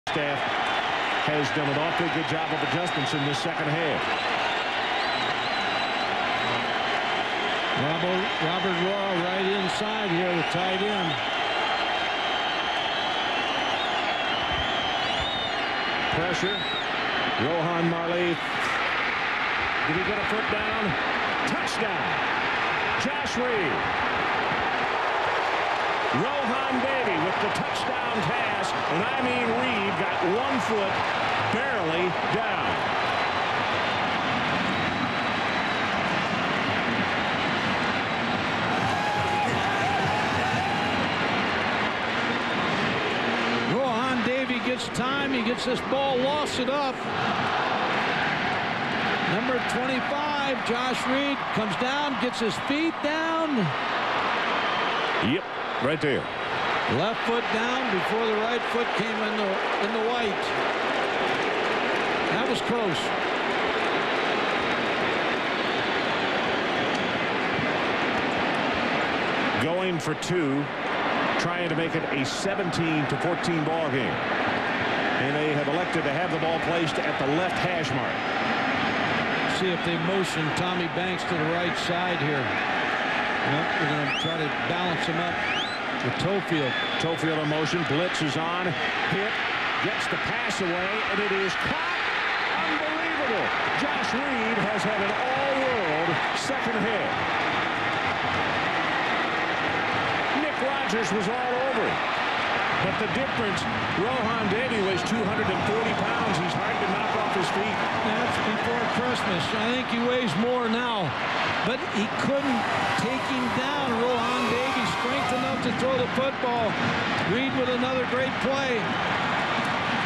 One of The Greatest TD & 2PC Series - LSU vs Georgia Tech 2000 Peach Bowl Amazing
One of The Greatest TD & 2 Point Conversion Plays - LSU vs Georgia Tech 2000 Peach Bowl Amazing Rohan Davey Josh Reed